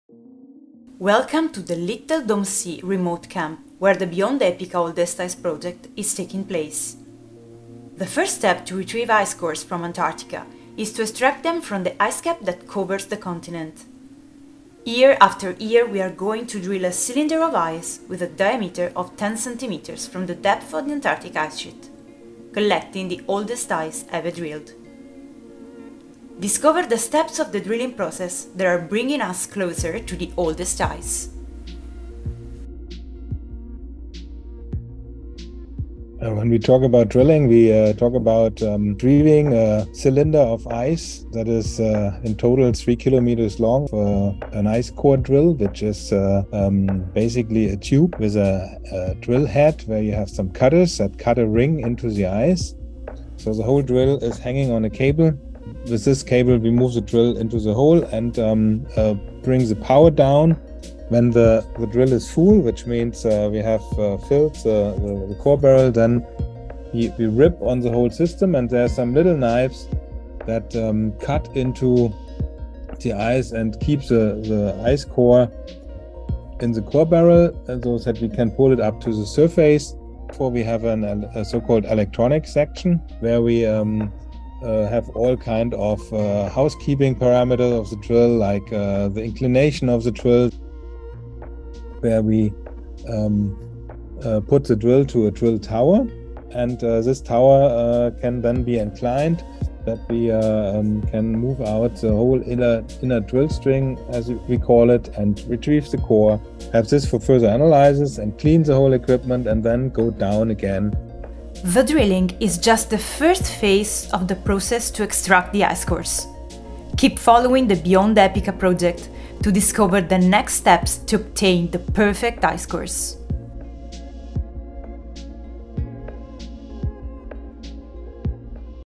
Beyond EPICA Storytelling | 7 | How to drill an ice core
The first step to retrieve ice cores from Antarctica is to extract them from the ice cap which covers the continent. Welcome to the remote camp of Little Dome C, where the Beyond EPICA Oldest Ice project is taking place.
Year after year we are going to drill a cylinder of ice with a diameter of 10 cm from the depth of the Antarctic ice sheet, collecting the oldest ice ever drilled.

Discover with us the steps of the drilling process taking us closer to the Oldest Ice.

Interview: Frank Wilhelms, Alfred Wegener Institute Helmholtz Centre for Polar and Marine Research | AWI